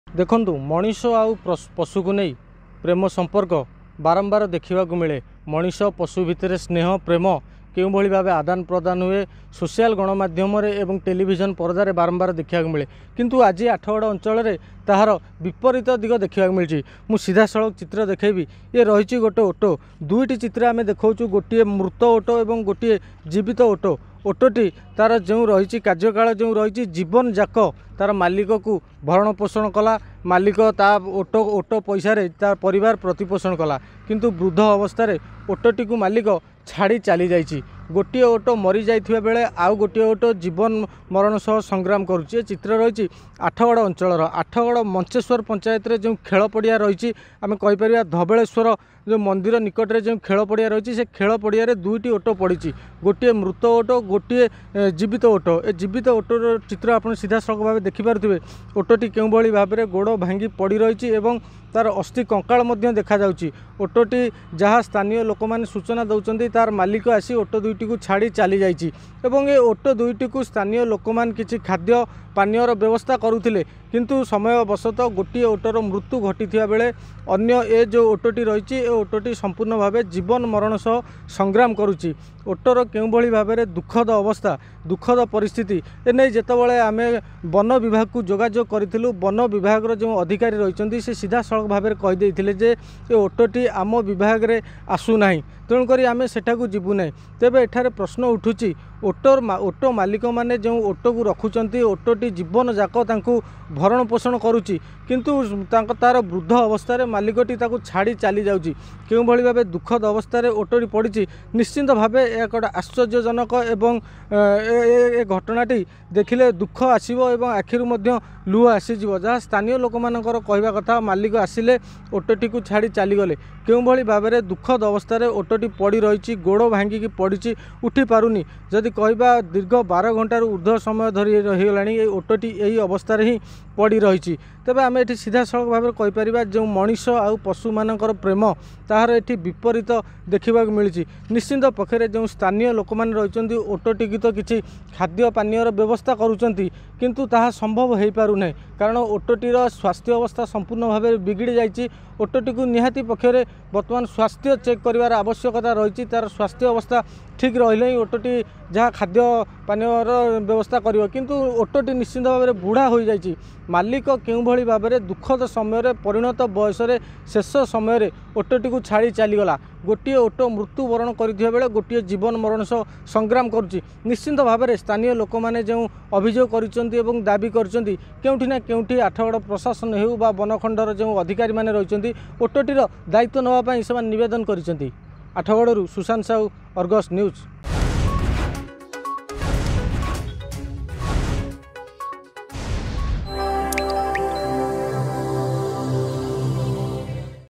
Cuttack: Inhuman owner leaves his aged Camels to die in Pain although they had sustained his family
Inhuman owner leaves his aged Camels to die in Pain although they had sustained his family in Cuttack's Athagarh.